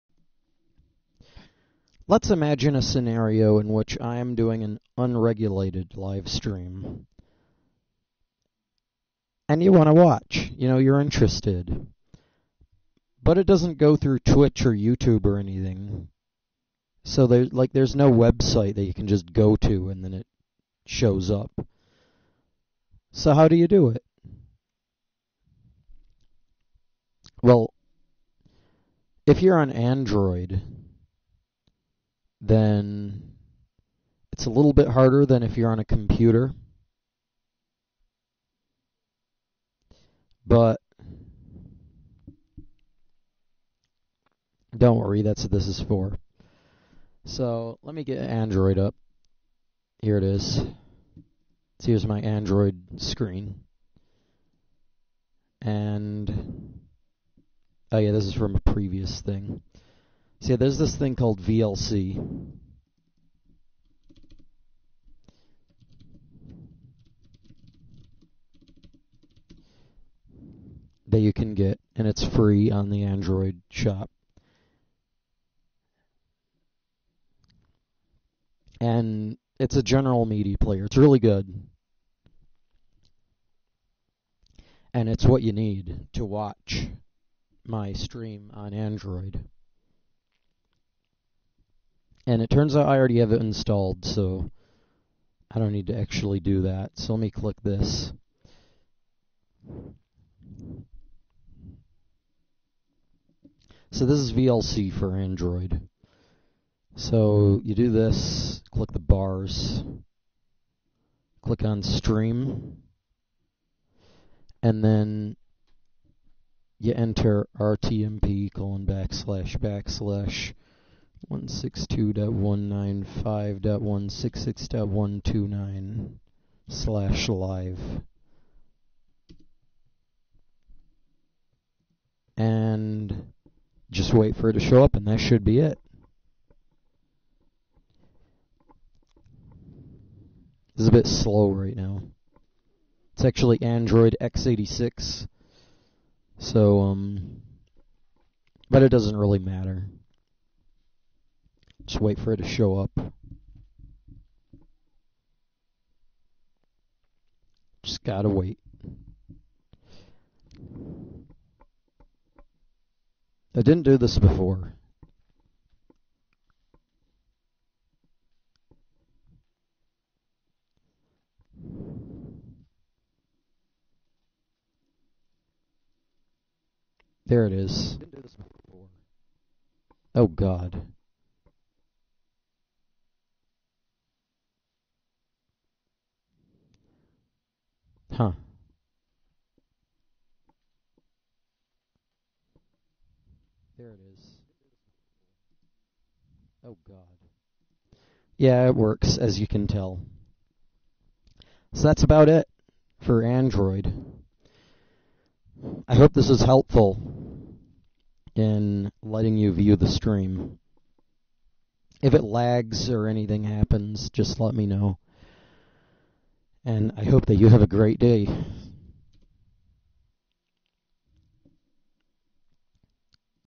Installing VLC Media Player and watching RTMP streams on Android
A brief segment on getting VLC Media Player, and then playing an RTMP stream.

The 3rd of a 3 part series of tutorials on how to view my RTMP streams.